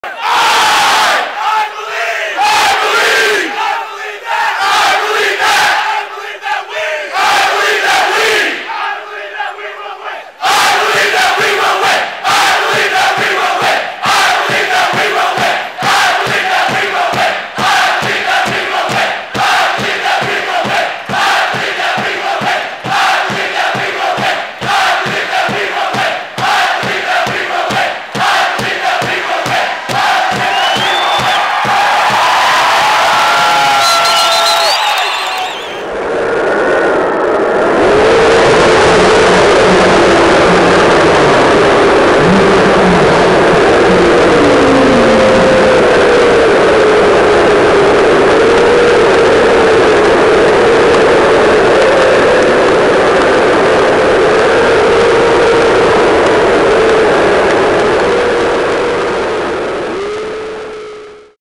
Believe... Aggie Basketball
The crowd at the Dee Glen Smith Spectrum is second to none.  Aside from the brilliance of Stew Morrill and our talented players, the fans here are a huge factor in the success of our Aggie Basketball program.